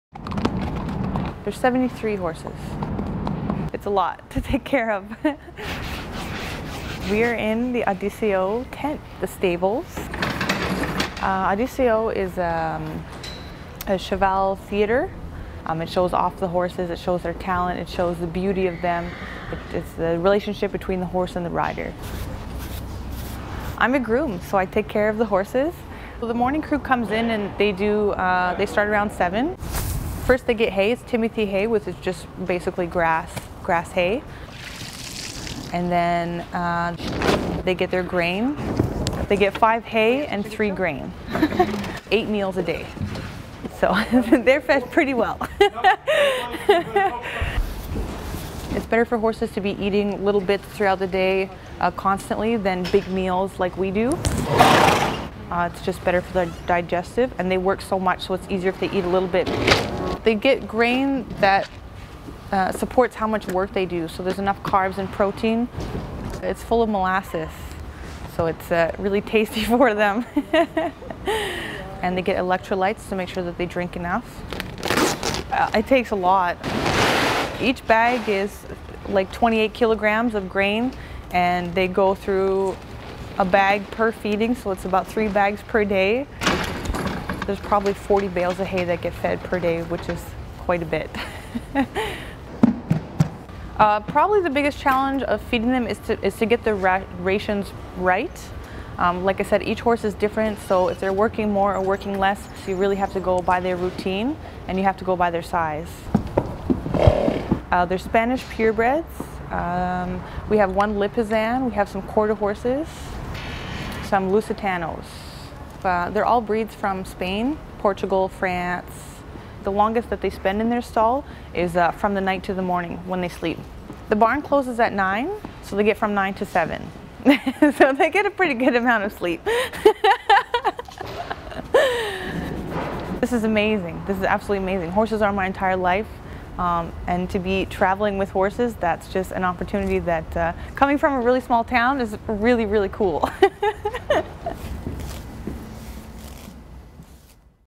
Before Dawn: Feeding the Cavalia horses
A groom with Cavalia takes us through a morning feeding routine of the show's horses, in Montreal, Tuesday June 16, 2015.  (Phil Carpenter / MONTREAL GAZETTE)